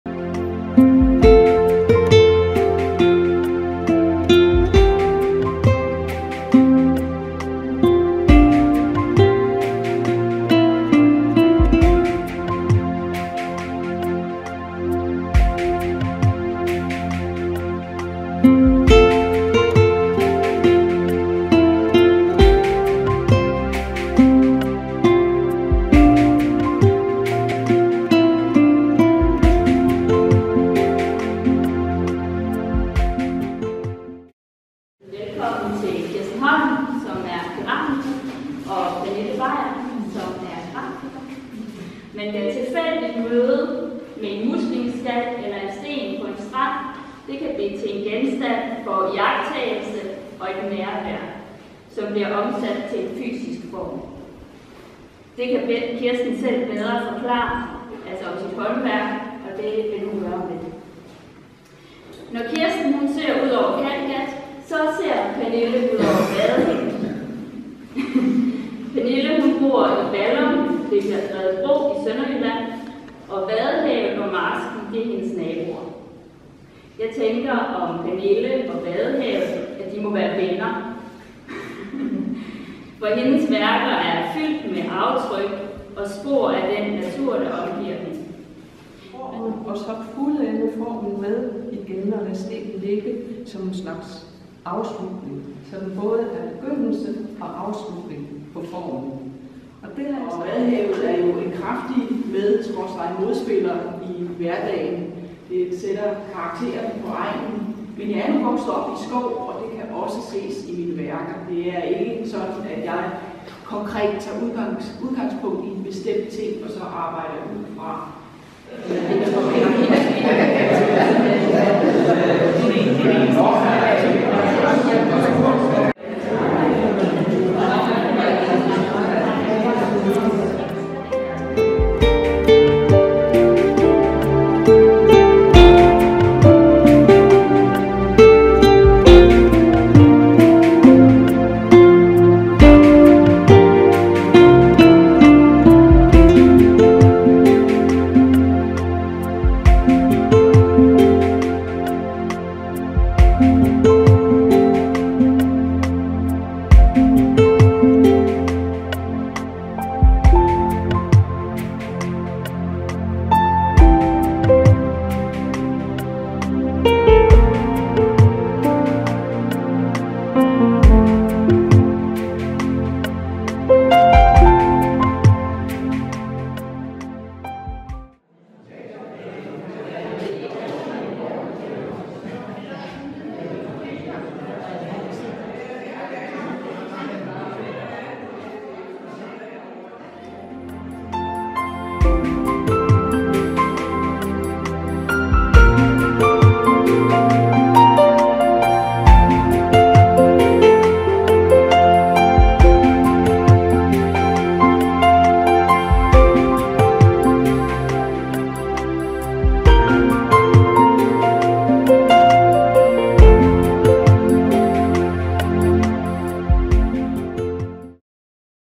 Kunstudstilling 2016 Ulstrup Slot
Hvorslev Kunstforening afholdte udstilling d. 30 april og d. 1. maj 2016, på Ulstrup Slot med Grafiker Pernille Bejer og Keramiker Kirsten Holm.